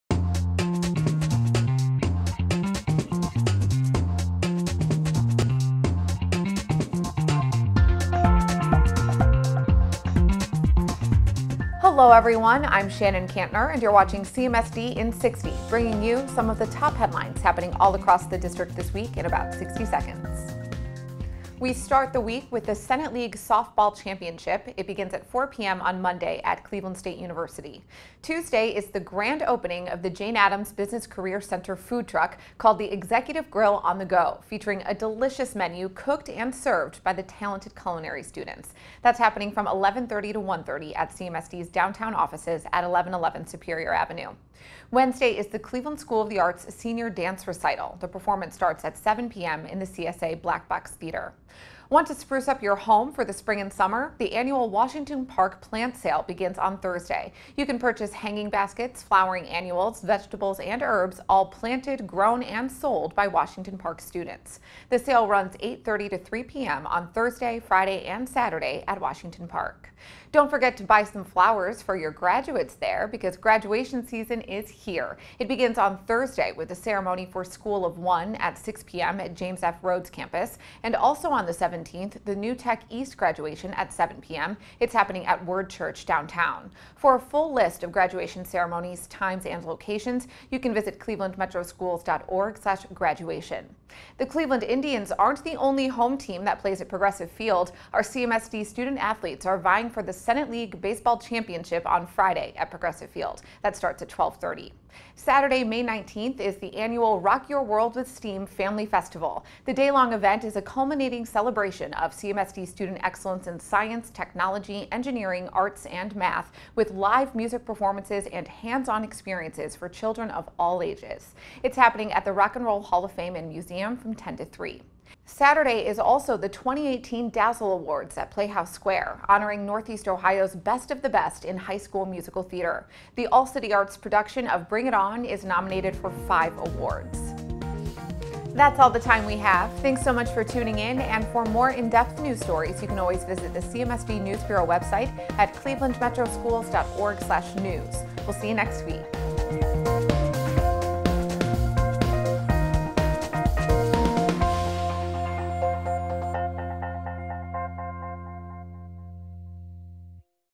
CMSD in 60 - Week of May 14
Want to know what's happening this week at CMSD? "CMSD in 60" brings you some of the top headlines from around the District in just 60 seconds. The weekly program is posted every Monday morning to help the community stay informed and look forward to the many activities happening across CMSD.

This week: Softball Championship, Executive Grille on the Go, CSA Senior Dance Recital, Washington Park Plant Sale, 2018 Graduations, Baseball Championship, Rock Your World with STEAM, Dazzle Awards